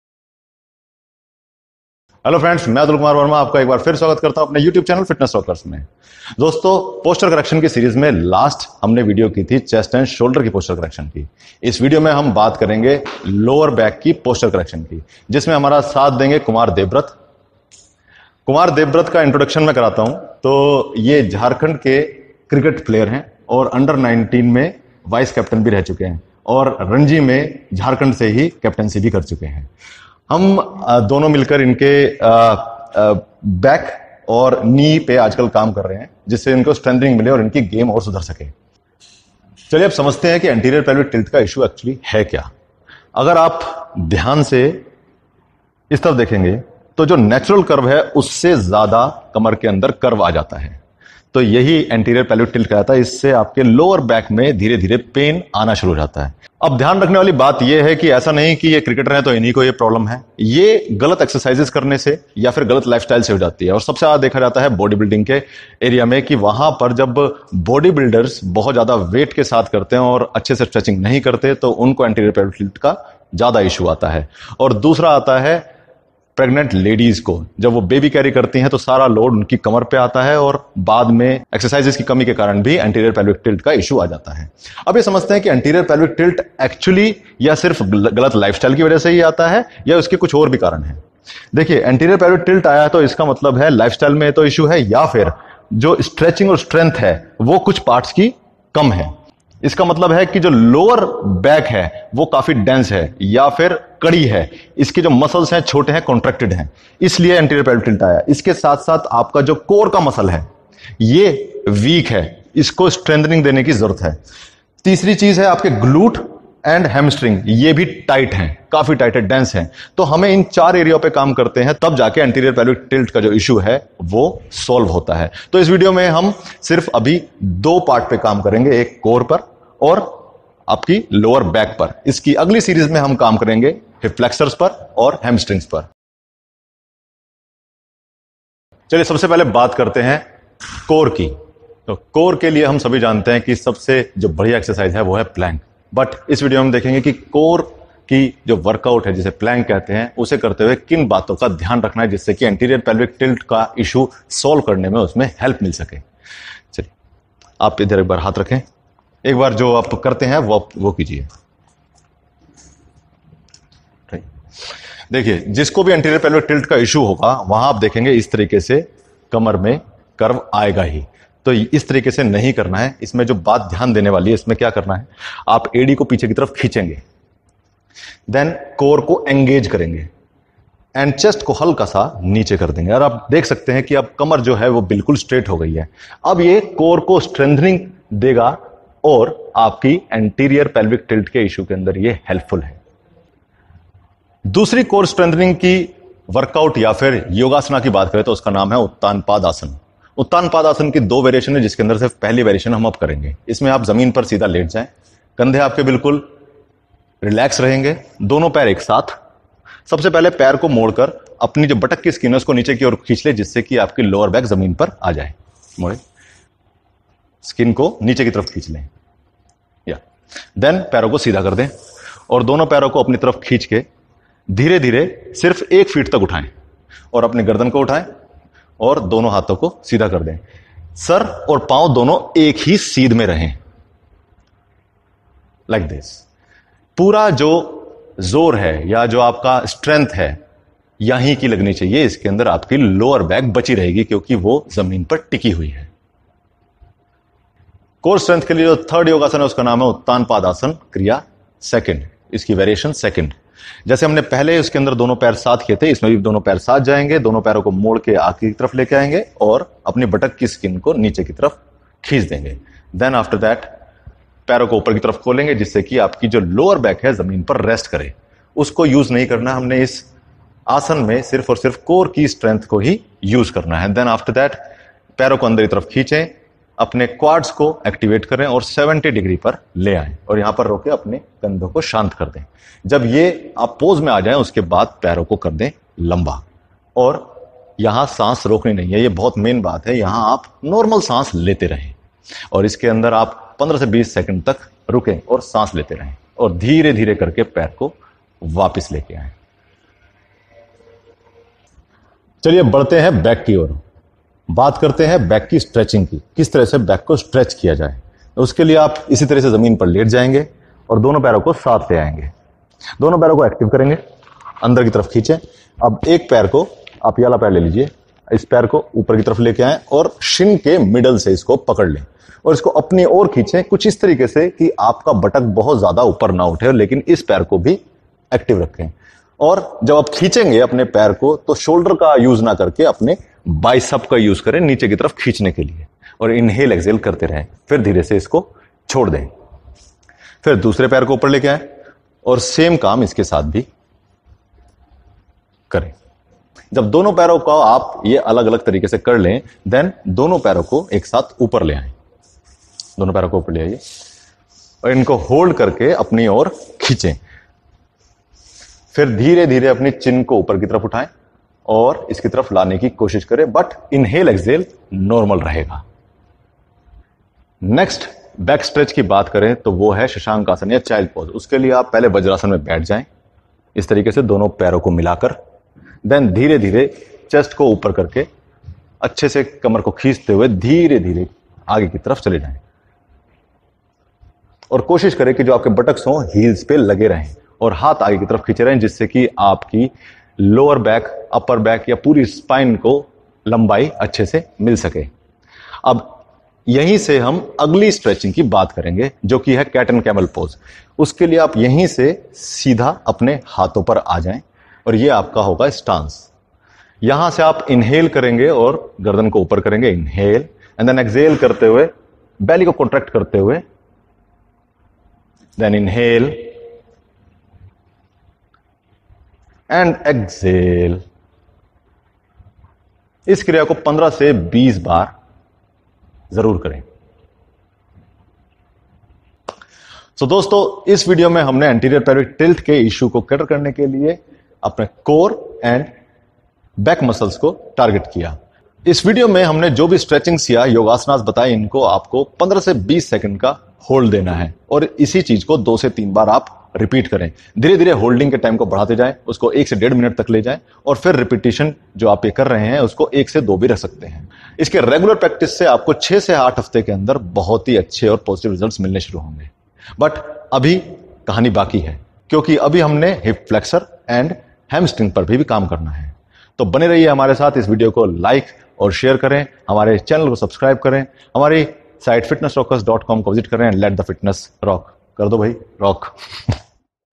How to fix Anterior Pelvic Tilt (APT) | Yoga & exercises for Lower Back Pain at home | Hindi
How to fix Anterior pelvic tilt or APT. Yoga, stretches & exercises for Anterior Pelvic Tilt correction, bad posture & lower back pain at home.

Bodybuilders who lift heavy weights with wrong technique, people who sit for long time, Pregnant women and girls wear high heals generally have this issue of Anterior pelvic tilt or APT. Its not limited to them only lack of exercises may also bring this issue. In long run a person having Anterior pelvic tilt or APT start having lower back pain.

In this video, I’m going to show you how to reverse the effects of chronic sitting by addressing what muscles are causing anterior pelvic tilt and how to strengthen the areas that are weak with the help of Yoga & exercises. Yoga is best to cope up with these posture imbalance. With a step by step attack plan, you are going to be able to fix this muscle and joint imbalance once and for all.

To start, it is important to define what anterior pelvic tilt or APT really is. It is the resultant posture of tightness and weakness in certain muscle groups of the core and pelvis that causes your pelvis to tilt anteriorly and downward. This can be seen pretty easily from the side in somebody that has it. You will notice an exaggerated curvature in the lower back and possibly even a protruding stomach. The belly is not protruding because of it being overweight (although it might be) but rather because the muscles of the abs are not strong enough to counteract the downward pull of the pelvis.

Realizing that our bodies work in a cross pattern muscularly, you will quickly see that the two areas of tightness are the lower back on the posterior side and the hip flexors or iliopsoas on the anterior or front side. The muscles of the hip flexors can become easily shortened because of the fact that they are in this state for prolonged periods of time from sitting. Whether you sit at work all day or just spend a long time in front of the computer or even driving, your hip flexors will have a tendency to adaptively shorten.

This causes a resultant weakness in the muscles on the opposite side of the joint, the glutes and hamstrings. When this happens you actually get a worsening of the problem since you don’t have the muscular strength to counteract the anterior and downward pull of the pelvis by the hip flexors. As a compensation to the glutes being weak, the lower back attempts to kick in and help out. It provides extra extension not provided by the glutes and in doing so, compromises its strength and integrity.

When combined with the pull of the iliopsoas on the lumber vertebrae, you get an overly active and tight low back. This not only can cause pain but further exacerbates the problem. Along with this static positioning of the pelvis you get a disuse stretch weakness on the abdominals. In order to counteract this cascade of problems you need to stretch the hip flexors, strengthen the abs with exercises that don’t over activate the hip flexors, and strengthen the glutes and hamstrings.

In this video we are going to demonstrate the yoga exercises to stretch and strengthen tight and week muscle groups respectively. Kumar Deobrat who is a sportsman and play cricket from Jharkhand team helping us in demonstrating these yoga asanas with Yoga expert/trainer Atul Kumar Verma at Gravity Health Club, Shakti Nagar under camera work & direction of Mr. Arjun Wadhawan.